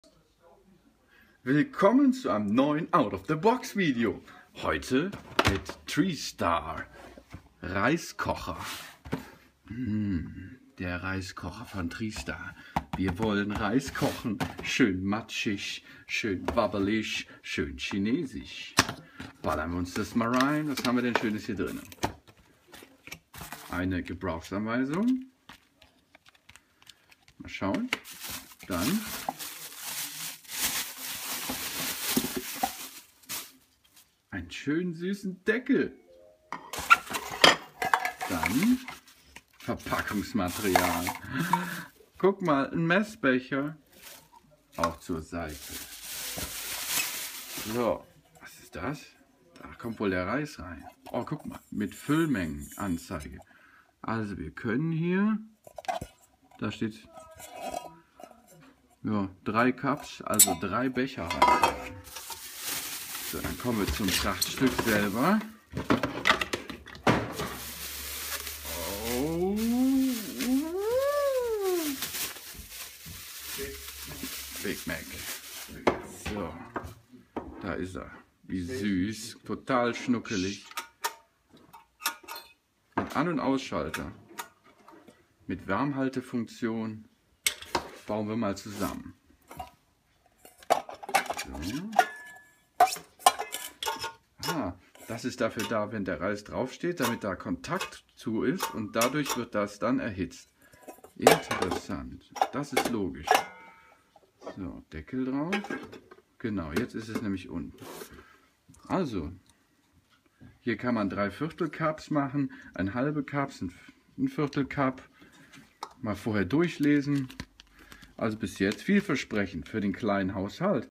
Tristar Reiskocher RK-6103 out of the box Unboxing
Zutaten:

Tristar Reiskocher RK-6103

Fassungsvermögen: 0,6L

Fazit: nicht so toll, da kein Timer, sehr klappriges Gerät. Da kann ich den Reis auch wieder im Topf  kochen.

pduesp